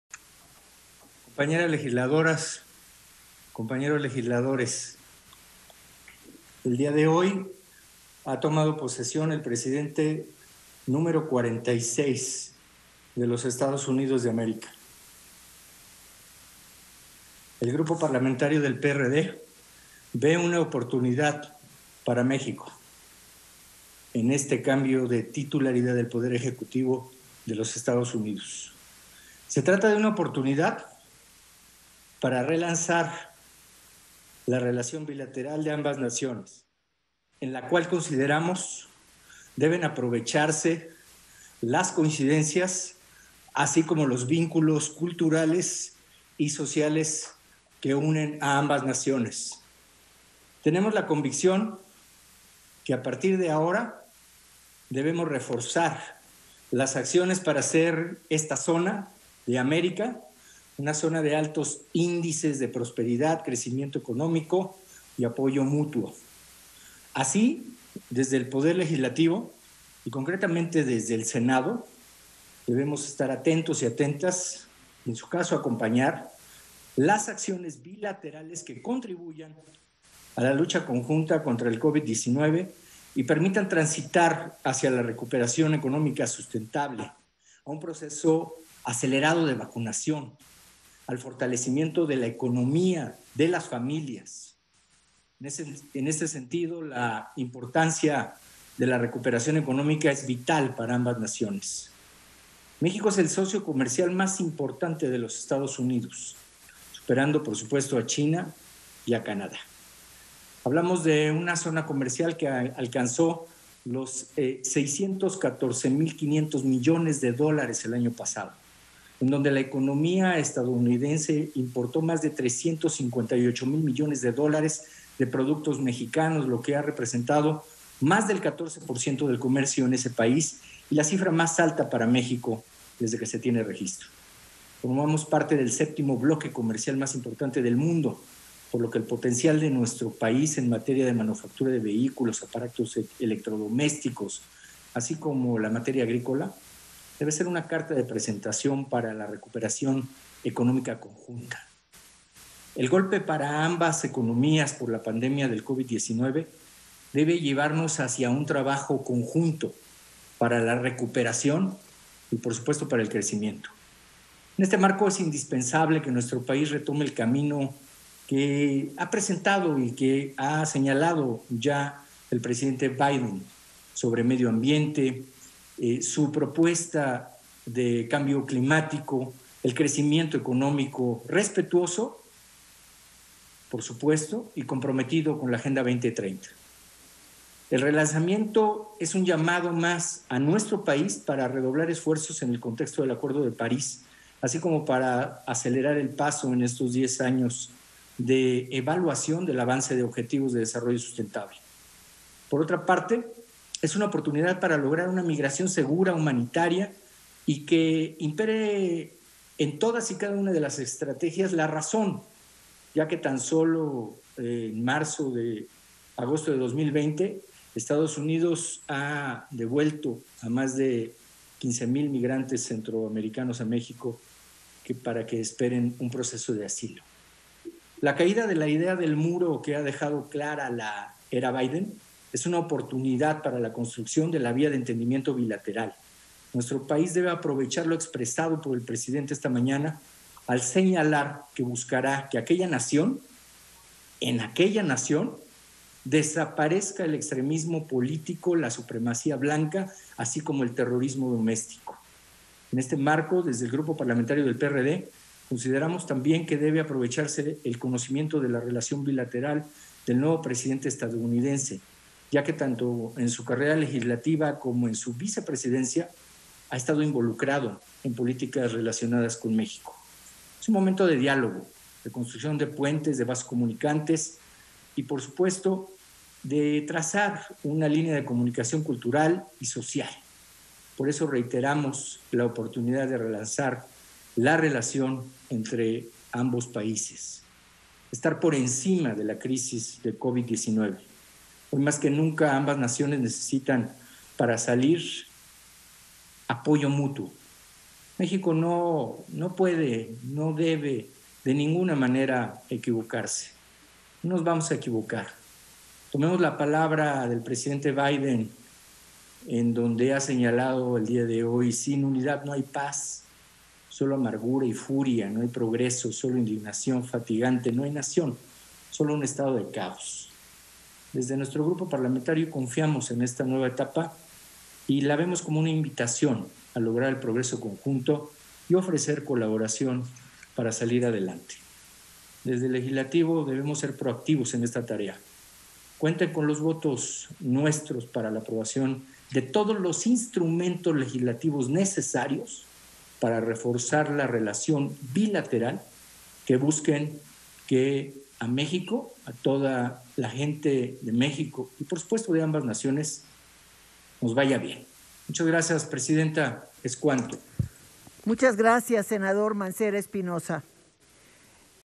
Cambio de poder en Estados Unidos, oportunidad para relanzar relación bilateral: Sen. Mancera (PRD)
El senador Miguel Ángel Mancera, del PRD, destaca que con el cambio de administración se deben aprovechar las coincidencias entre nuestros países para trabajar conjuntamente hacia la recuperación y el crecimiento. Añade que es una oportunidad para lograr una migración segura y humanitaria.

Lo anterior, al referirse a la relación bilateral con los Estados Unidos de América.